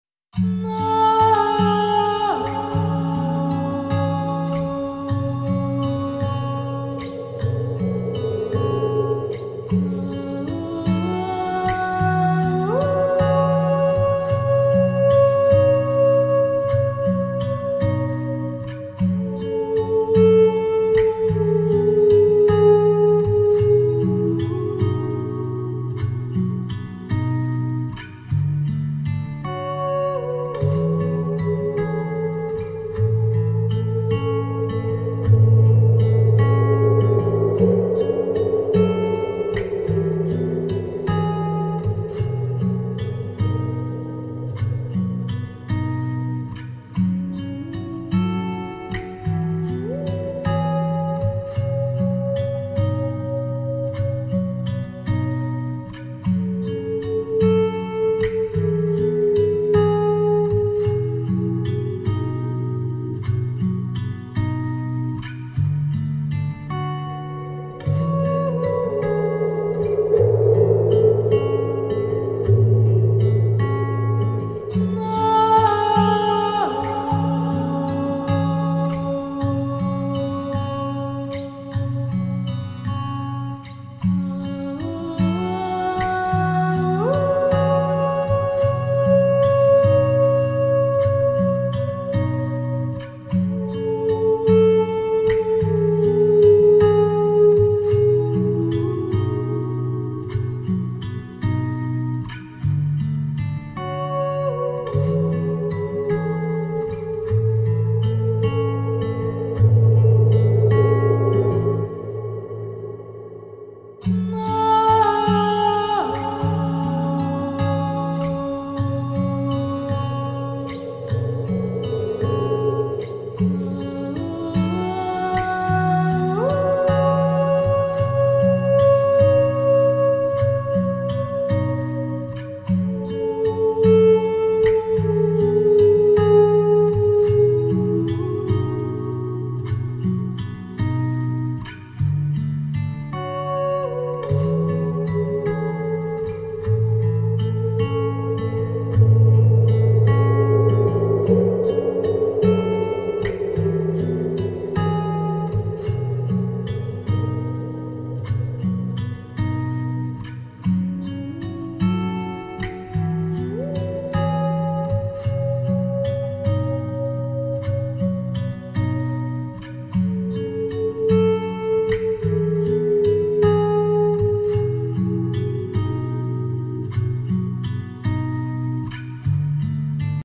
Magical Fairy Singing for a Calming and Soothing Atmosphere
Experience the enchanting world of fairies with this beautiful vocal performance. Let the soothing sounds of the magical fairy's singing create a calming and peaceful atmosphere, helping you relax and unwind from the stress of everyday life.

This video features a stunning visual display of a fairy forest, bringing you closer to the magical realm of fairies. The calming melodies and angelic voice of the fairy will transport you to a place of serenity and tranquility.

Perfect for meditation, relaxation, and stress relief, this video is a must-watch for anyone seeking a peaceful and calming escape.